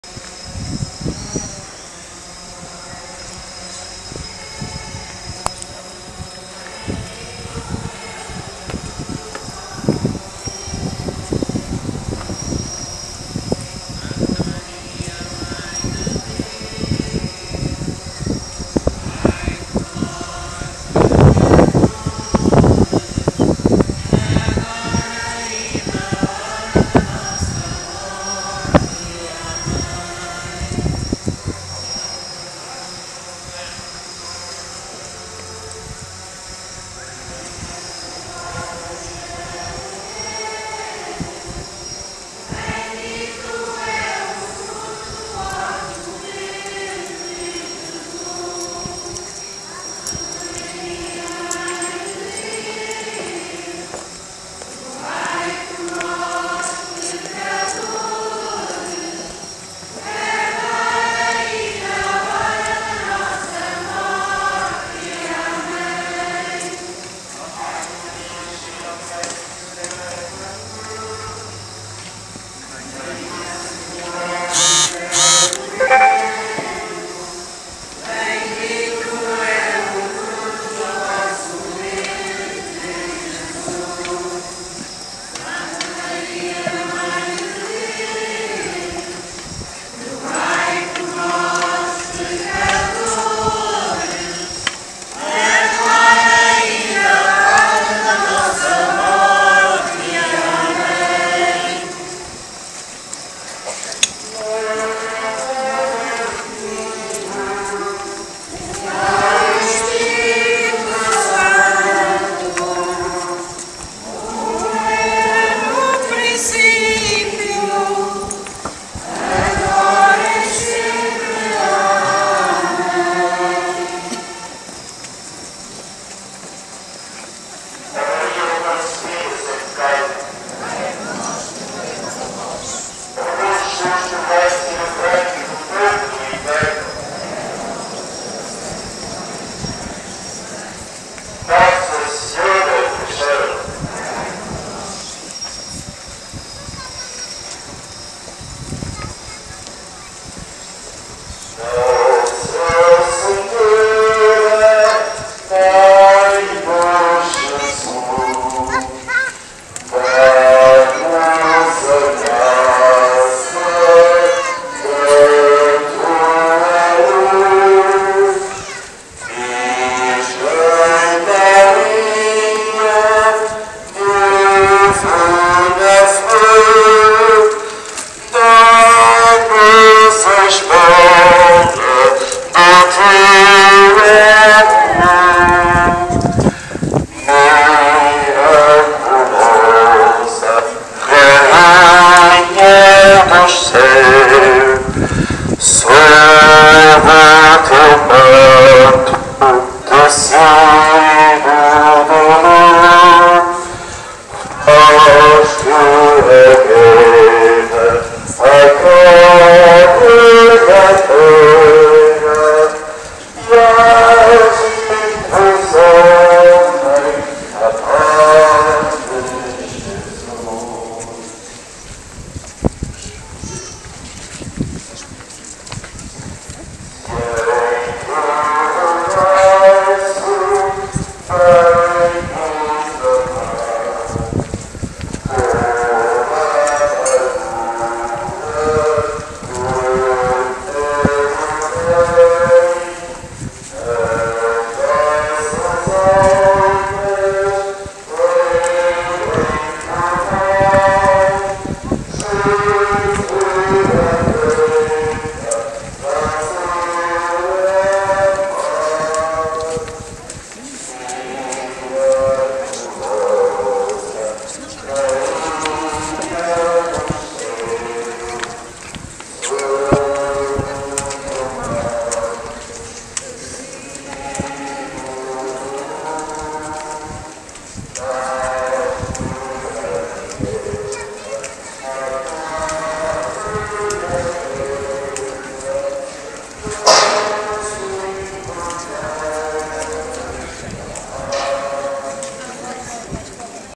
Arcos de Valdevez: Procissão de Velas da Sra. do Castelo 2017
A Procissão de Velas da Sra. do Castelo, em Arcos de Valdevez, na quinta-feira à noite